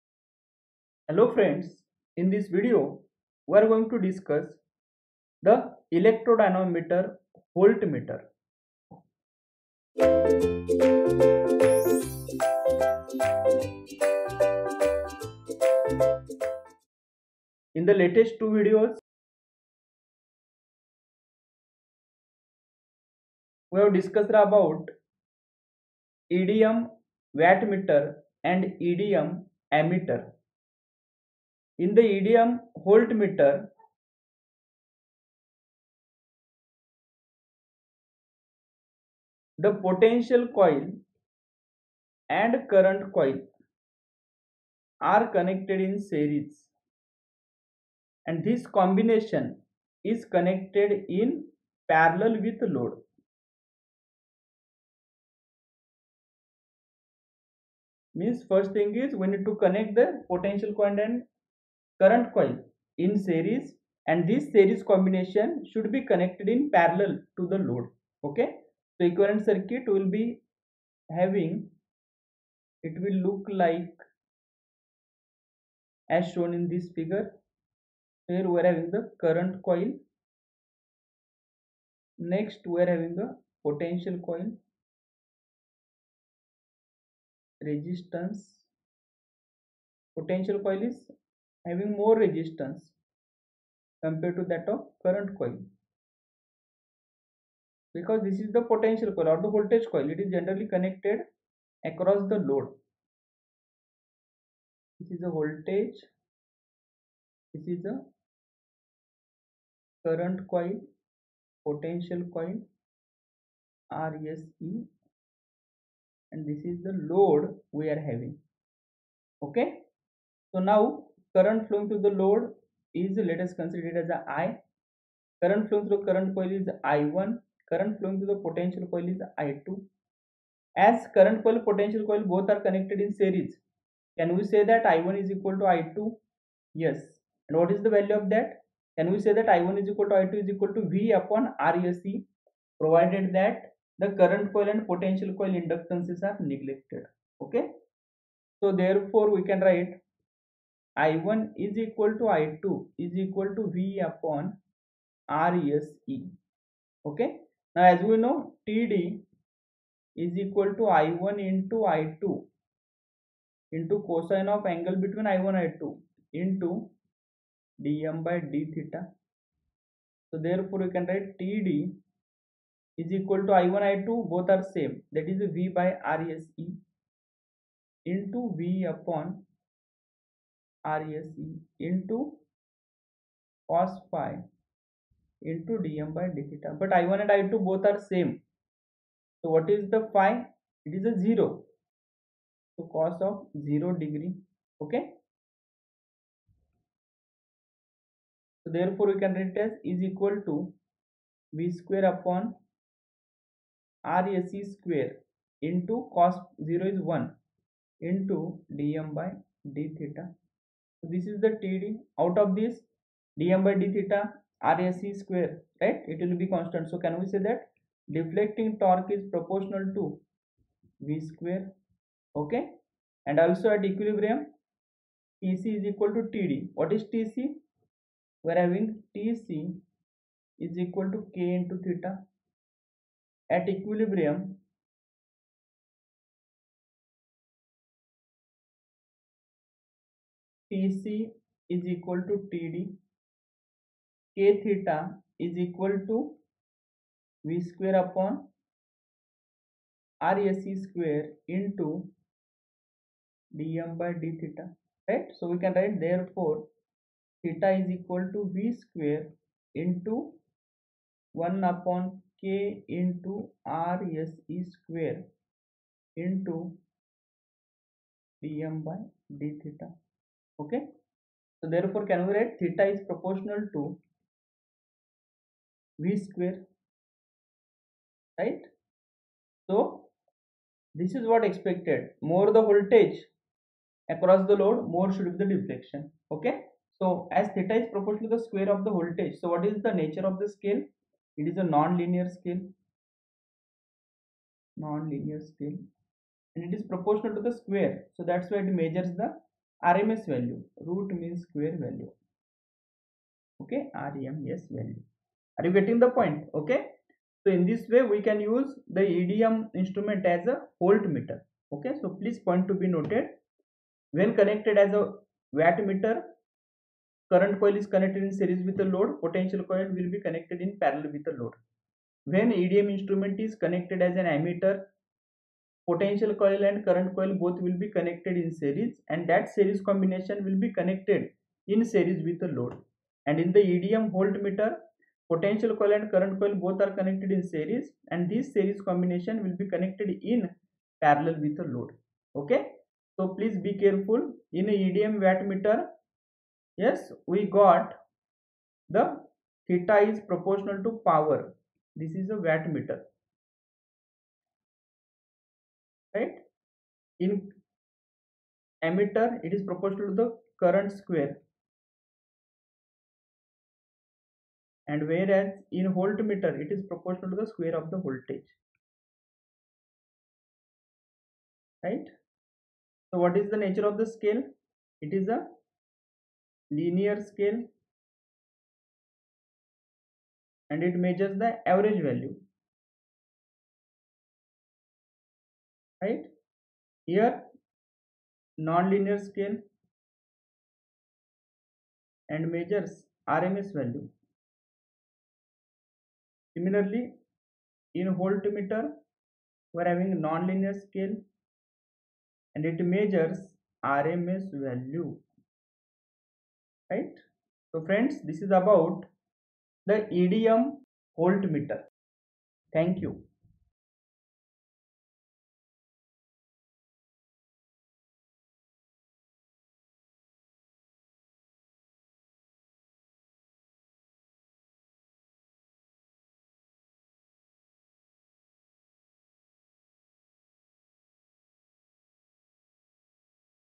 Electrodynamometer Voltmeter | Electromechanical Indicating Instruments | GATE IE
Explore the world of Electrodynamometer Voltmeters with our latest video on Electromechanical Indicating Instruments for GATE IE preparation. Dive into the intricacies of this crucial topic in electrical engineering. Learn about its applications, functions, and how it plays a role in GATE Theory. Whether you're a student or a curious mind, this video provides valuable insights. Simplifying complex concepts, we make studying enjoyable and effective. Join us on this educational journey and boost your understanding of Electrodynamometer Voltmeters.

Happy Learning.